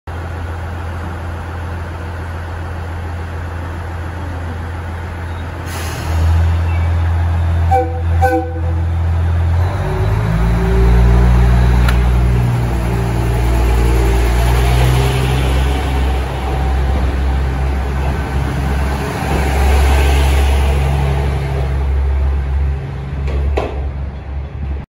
キハ110系 111-102編成 磐越東線(ゆうゆうあぶくまライン)普通ワンマン 郡山行き いわき駅発車
キハ110系って東日本そこら中で存在していますよね。飯山線や八高線、磐越西線等。
6番線の発車メロディは、全てワンマン化になった為発車スイッチを扱う列車は無いです。